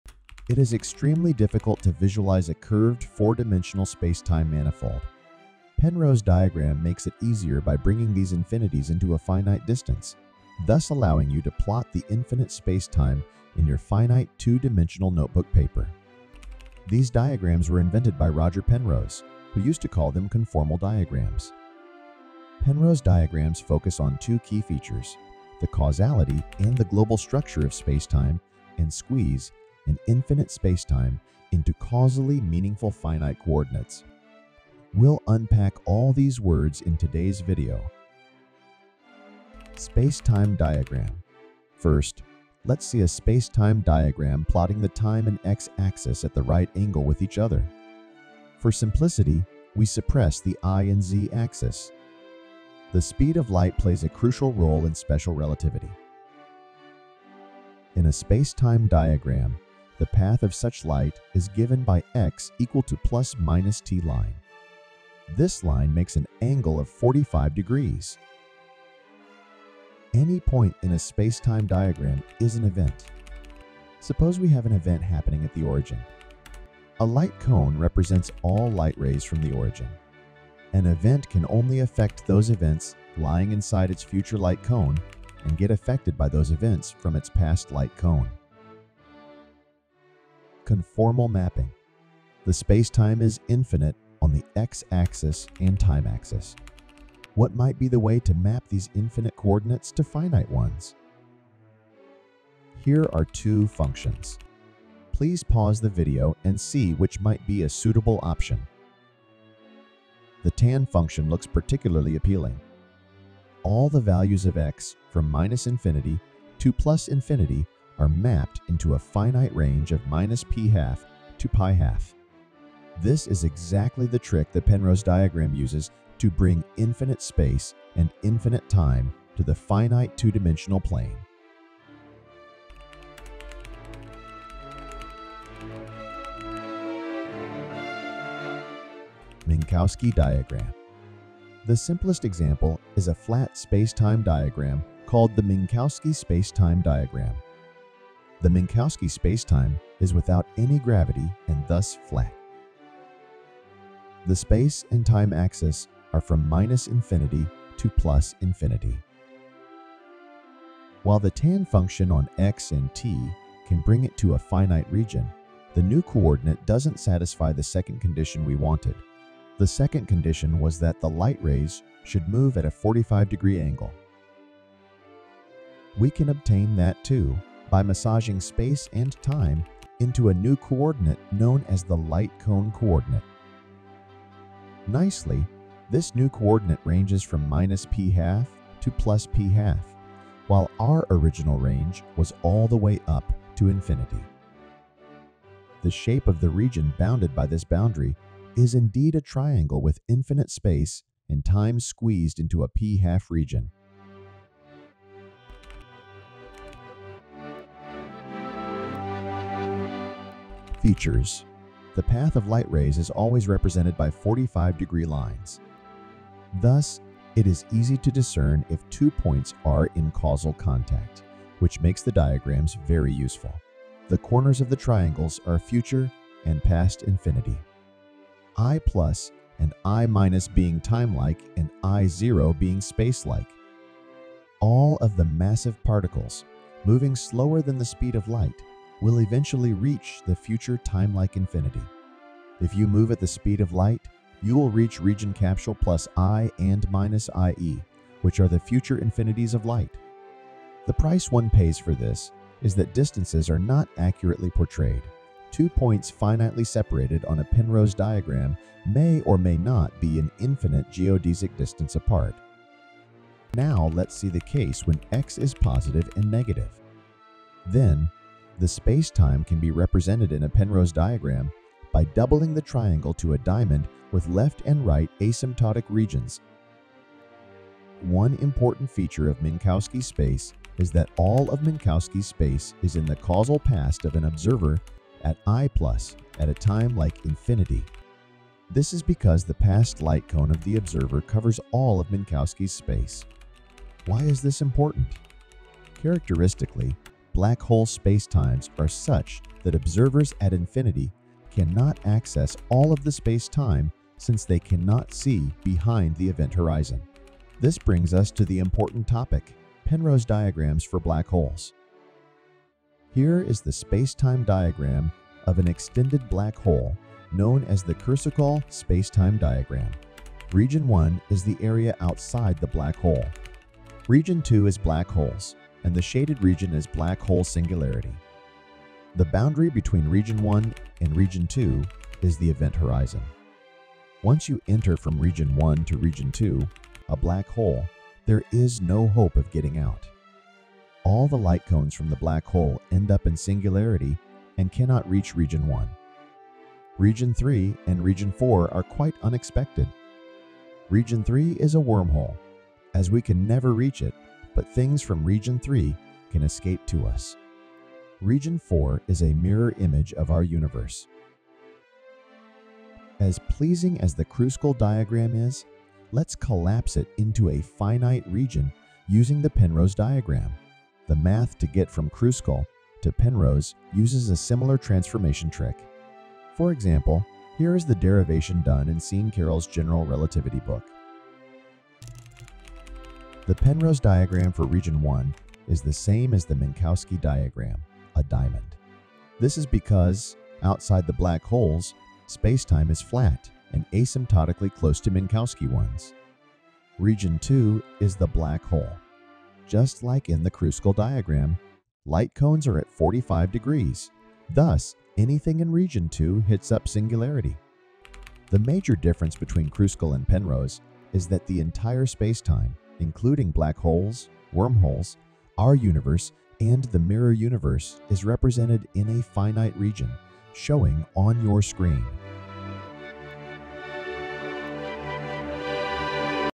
Penrose diagrams explained
It is extremely difficult to visualize a curved four-dimensional space-time manifold. The penrose diagram makes it easier by bringing these infinities into a finite distance, thus allowing you to plot the infinite space-time in your finite two-dimensional notebook paper. These diagrams were invented by Roger Penrose, who used to call them conformal diagrams. Penrose diagrams focus on two key features: the causality and the global structure of space-time and squeeze an infinite spacetime into causally meaningful finite coordinates – we’ll unpack all these words in today’s video!